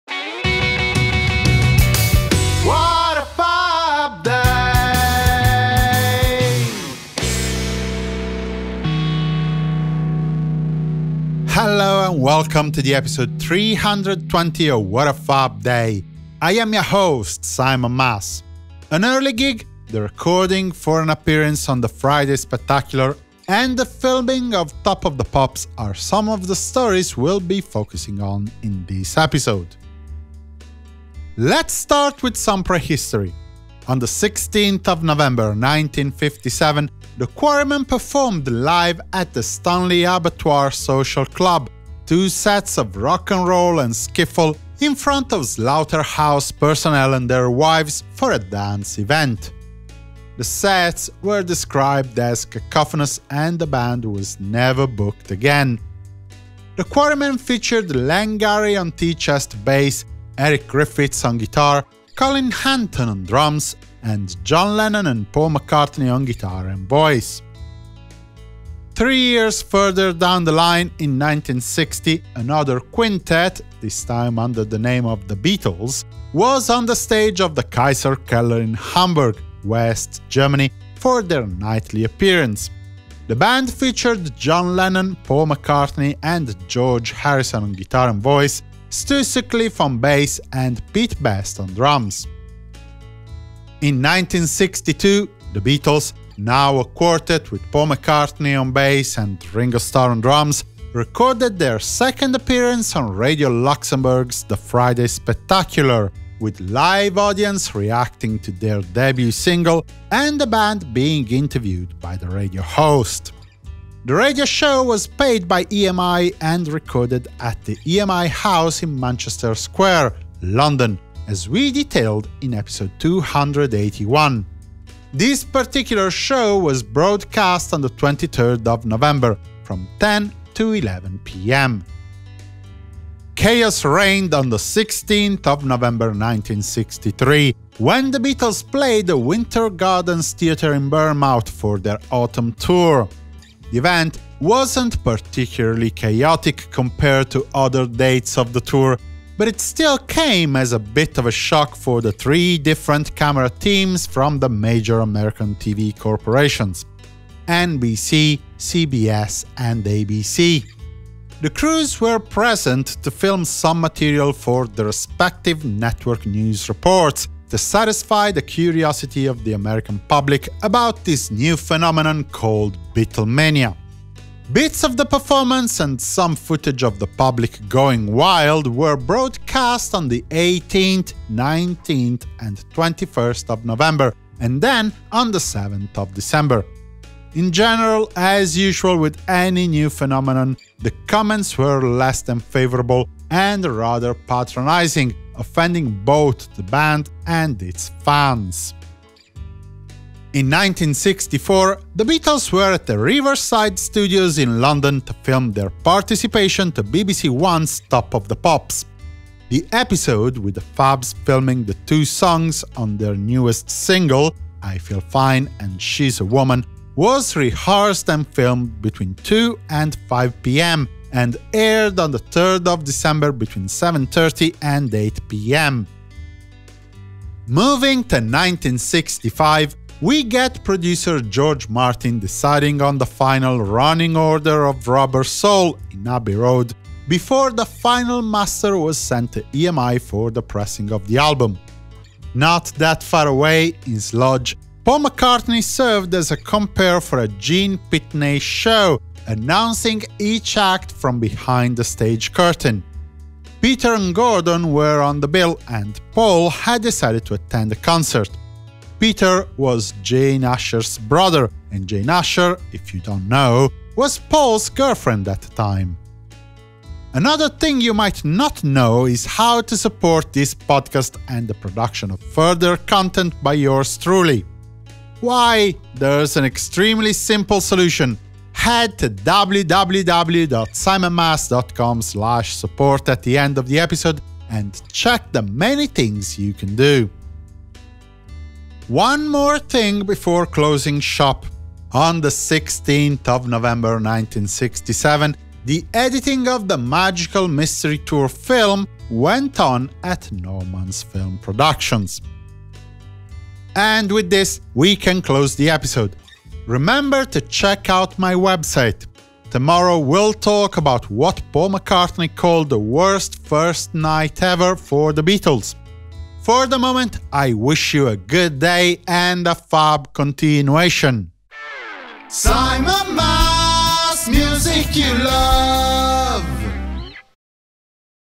Filming Beatlemania [ep. 320]
What happened in Beatles' history on the 16th of November? Listen to this podcast episode for an early gig, an appearance on The Friday Spectacular, one on Top Of The Pops... and more!

***CHAPTERS***
0:00 Introduction.
0:31 In 1957, the Quarrymen play at the Stanley Abattoir Social Club.
1:08 In 1960, the Beatles are on the stage of the Kaiserkeller in Hamburg.
1:33 In 1962, the Beatles record an appearance at The Friday Spectacular.
2:13 In 1963, the Beatles play the Winter Gardens Theatre in Bournemouth.
2:41 American crews film the concert.
3:20 In 1964, the Beatles film an appearance for BBC's Top Of The Pops.
3:47 In 1965, Paul McCartney acts a compere for a live show.
4:48 In 1967, the editing of Magical Mystery Tour goes on.
5:03 Conclusion.

***LINKS***
Please help me giving you even better content

Daniele Monterisi

Mariella Tarantino